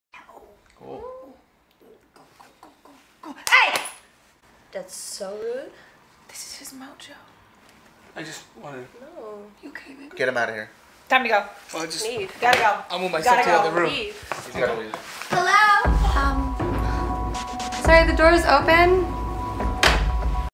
roommateTWAN PT2
Roommates
Starring Twan Kuyper, Adam Waheed, Spence, Nick Antonyan, CJ Franco, and Margo Graff
Comedy